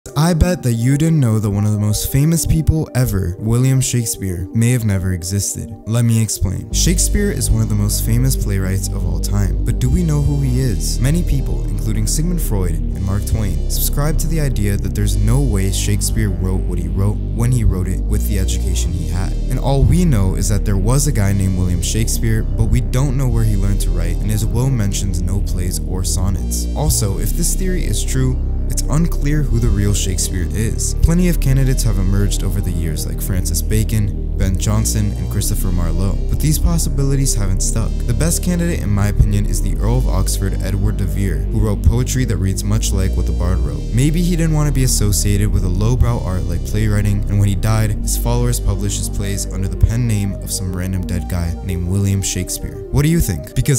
william shakespeare never existed???
hey it's me gabe (@gabesweats) from tiktok! in this video, i go over why William Shakespeare may have never existed.